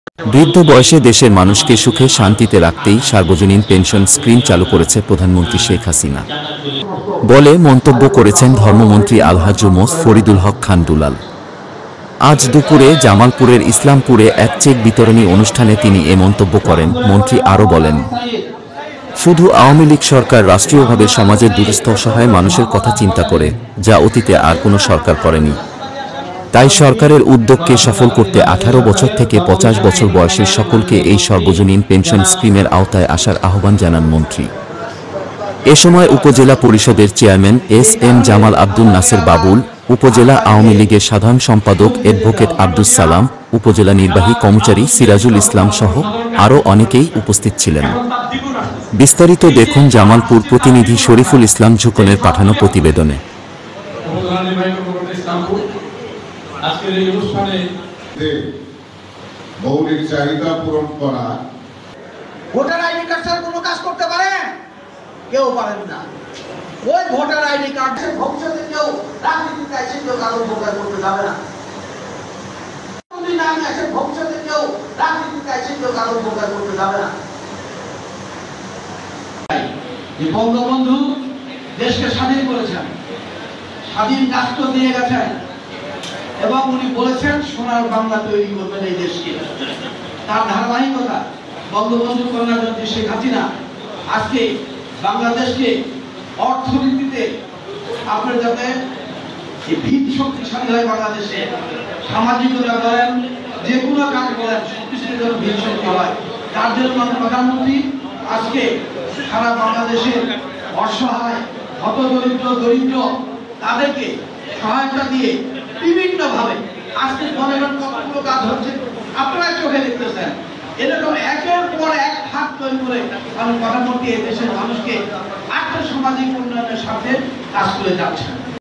বৃদ্ধ বয়সে দেশের মানুষকে সুখে শান্তিতে রাখতেই সার্বজনীন পেনশন স্ক্রীম চালু করেছে প্রধানমন্ত্রী
বৃদ্ধ বয়সে দেশের মানুষকে সুখে শান্তিতে রাখতেই সার্বজনীন পেনশন স্ক্রীম চালু করেছে প্রধানমন্ত্রী শেখ হাসিনা বলে মন্তব্য করেছেন ধর্মমন্ত্রী আলহাজ্ব মোঃ ফরিদুল হক খান দুলাল। আজ দুপুরে জামালপুরের ইসলামপুরে এক চেক বিতরণী অনুষ্ঠানে তিনি এ মন্তব্য করেন।

মন্ত্রী আরও বলেন, শুধু আওয়ামী লীগ সরকার রাষ্ট্রীয়ভাবে সমাজের দুঃস্থ-অসহায় মানুষের কথা চিন্তা করে, যা অতীতে আর কোন সরকার করেনি। তাই সরকারের উদ্যোগকে সফল করতে ১৮ বছর থেকে ৫০ বছর বয়সের সকলকে এই সর্বজনীন পেনশন স্ক্রীমের আওতায় আসার আহবান জানান মন্ত্রী।

এসময় উপজেলা পরিষদের চেয়ারম্যান এস এম জামাল আব্দুন নাসের বাবুল,উপজেলা আওয়ামী লীগের সাধারণ সম্পাদক এডভোকেট আব্দুস সালাম, উপজেলা নির্বাহী কর্মকর্তা সিরাজুল ইসলামসহ আরও অনেকেই উপস্থিত ছিলেন।